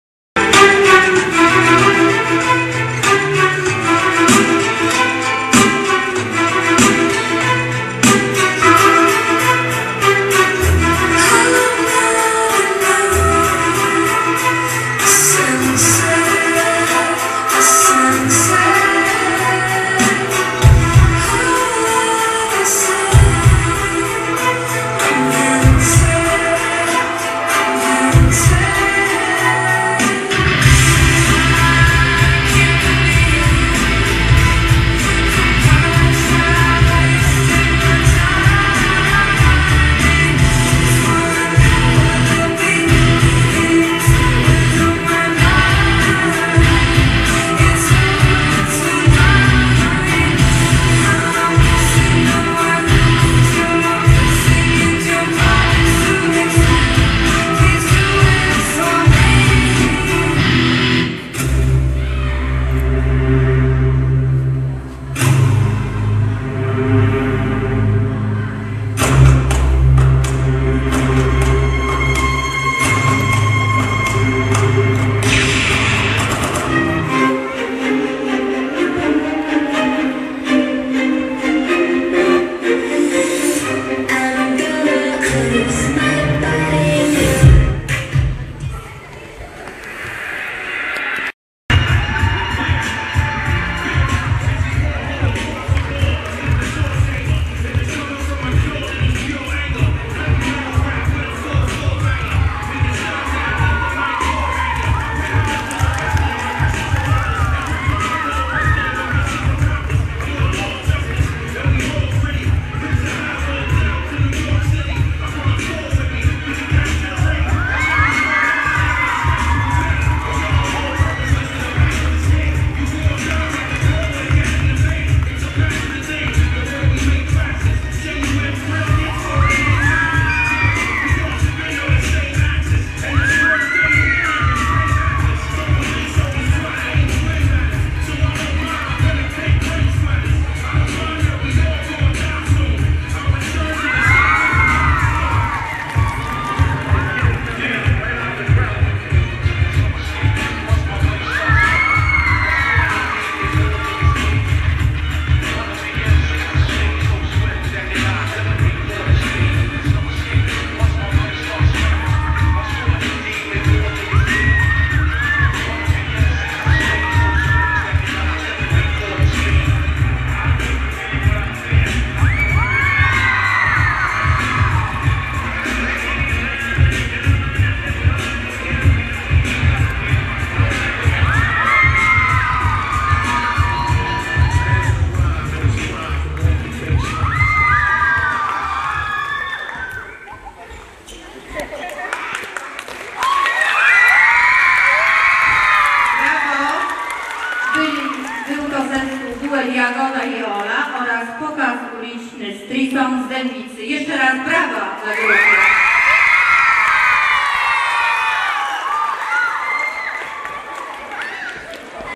Jagoda, Ola i grupa Street Zone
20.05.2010 - Pokaz duetu Jagoda i Ola, oraz grupy Street Zone podczas II Wojewódzkiego Przeglądu Amatorskich Dziecięcych Grup Tanecznych Pantofelek Królowej Jadwigi - Dębica 2010. Miejsce: Dom Kultury Mors - Dębica.